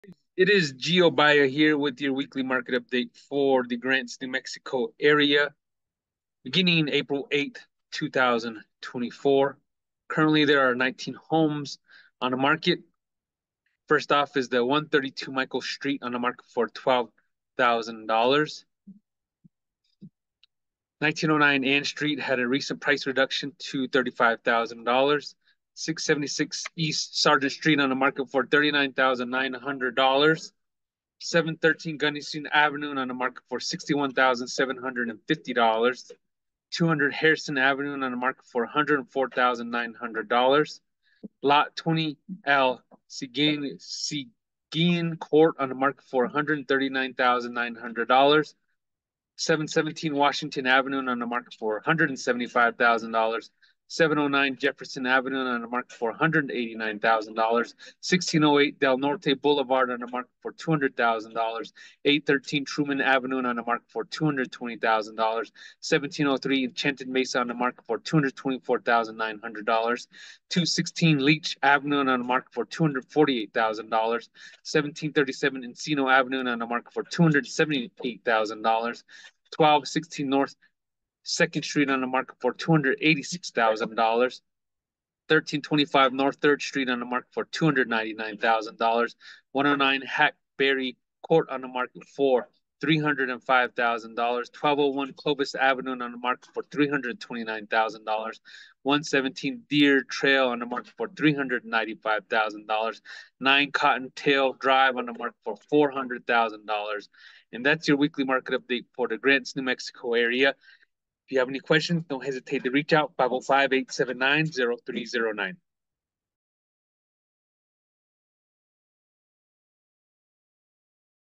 GRANTS MARKET UPDATE 4/8/2024
𝑴𝑨𝑹𝑲𝑬𝑻 𝑼𝑷𝑫𝑨𝑻𝑬! 𝑴𝑨𝑹𝑲𝑬𝑻 𝑼𝑷𝑫𝑨𝑻𝑬!

To keep you updated with the market, here is the updated listing of the properties in 𝑪𝒊𝒃𝒐𝒍𝒂 County in New Mexico.

If you're thinking of selling or buying a home, give us a call today.